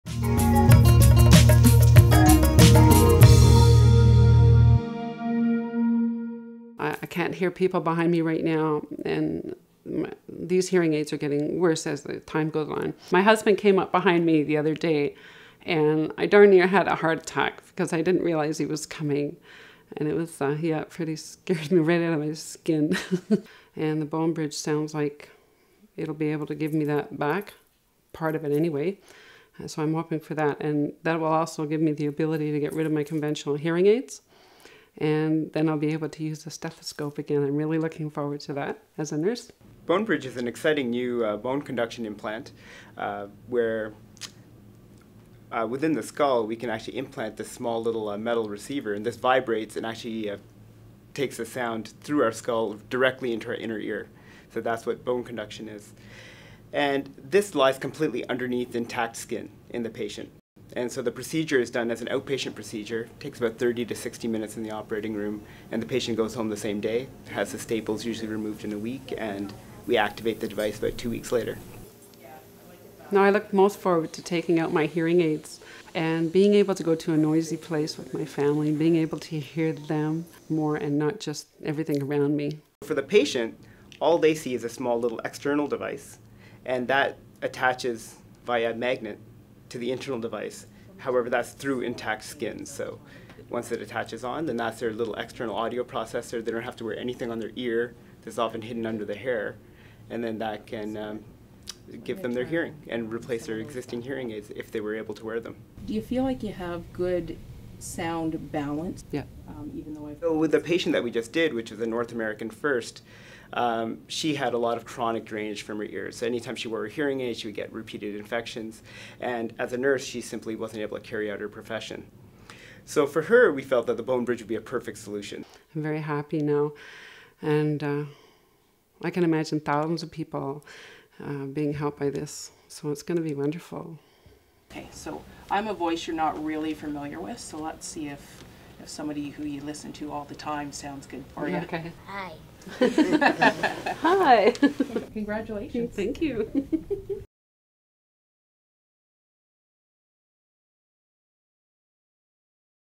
BONEBRIDGE bone conduction implant
On April 25, 2013, Kelly Dickson became the first patient in North America to receive the BONEBRIDGE™ -- a bone conduction hearing implant. The surgery was successfully performed at London Health Sciences Centre (LHSC) by Dr. Sumit Agrawal and his surgical team. The hearing device was activated on May 10, allowing Dickson to hear a full range of sounds.